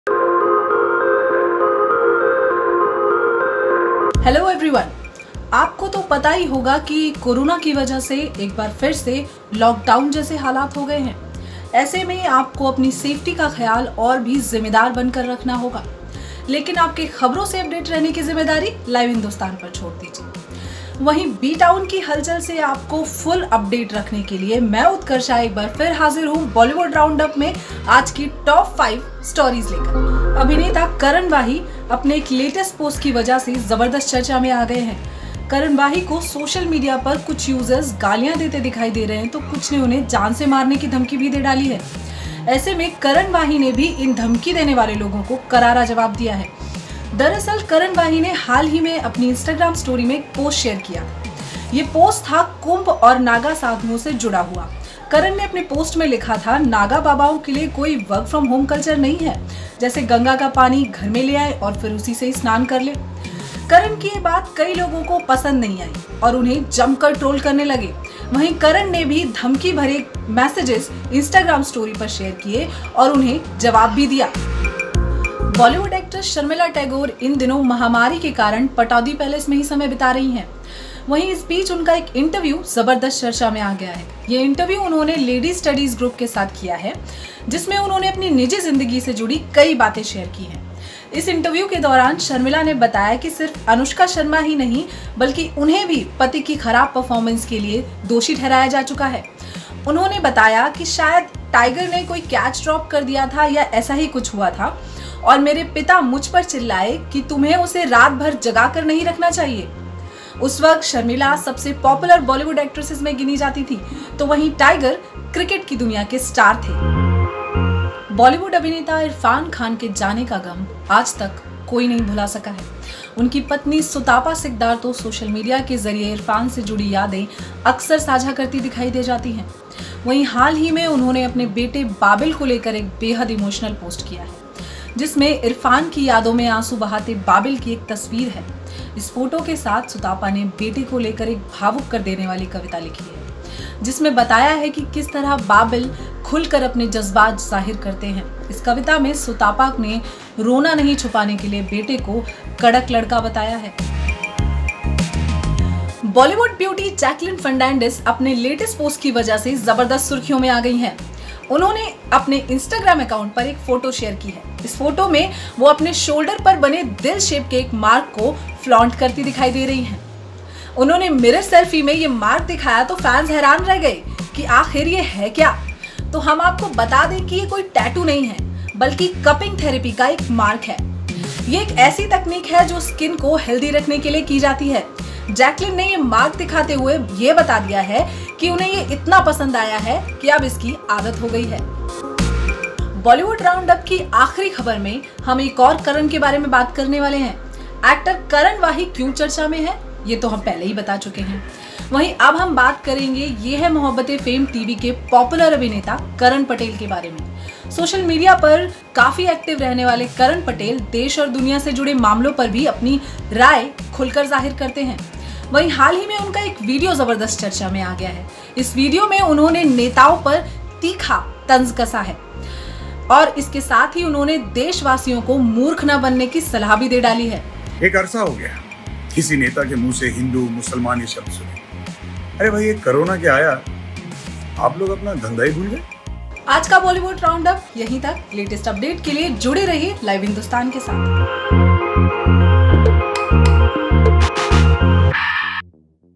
Bollywood Round Up: इस टीवी ऐक्टर को मिली जान से मारने की धमकी, जानें B-Town में खास
बॉलीवुड राउंडअप में जानिए आज कौन-कौन सी  खबरों ने बी-टाउन में मचाई जबरदस्त हलचल। जहां एक तरफ कुंभ और नागा साधुओं पर करण वाही का बयान चर्चा में रहा वहीं दूसरी तरफ एक्ट्रेस शर्मिला टैगोर के दिलचस्प किस्से लोगों का ध्यान खींचते नजर आए।